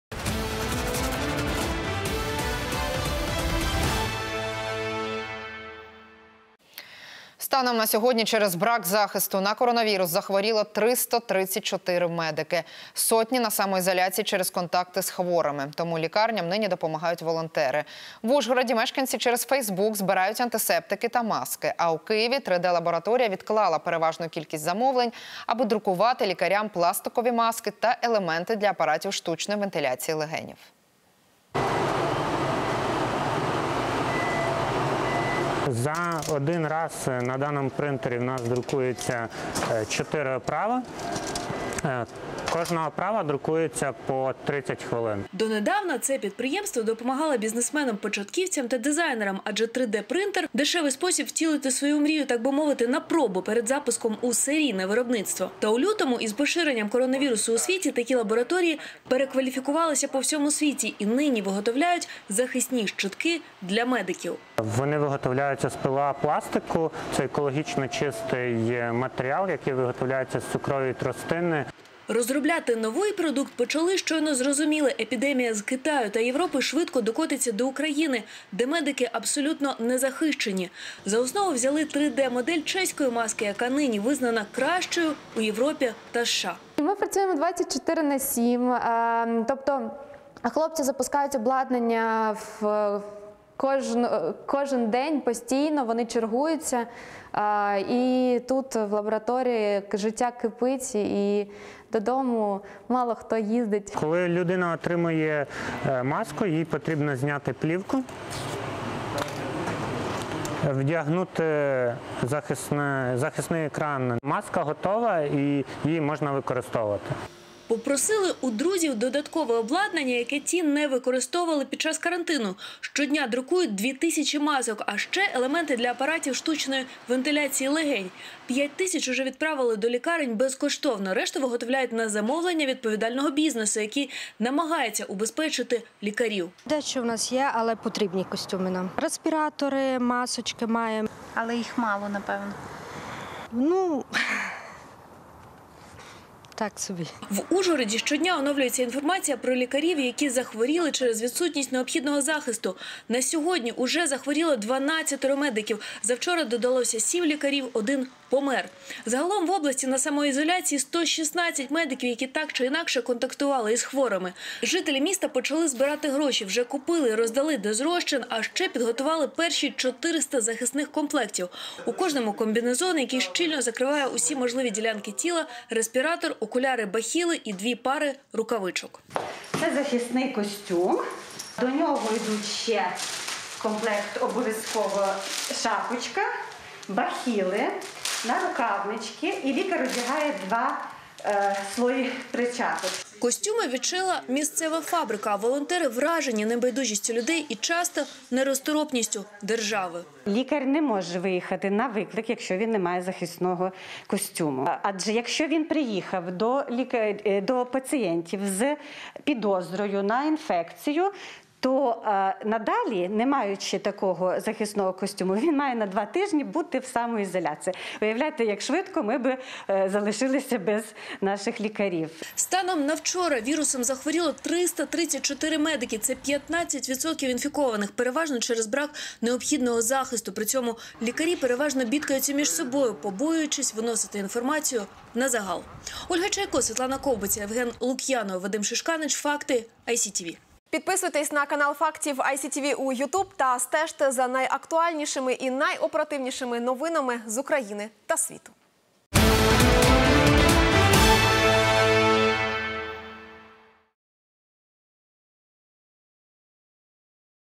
Маски на 3D-принтере и сбор антисептиков: как в Украине волонтеры помогают врачам
В Ужгороде жители через Facebook собирают антисептики и маски, а в Киеве 3D лаборатория отложила подавляющее большинство заказов, чтобы печатать врачам пластиковые маски и элементы для аппаратов искусственной вентиляции легких.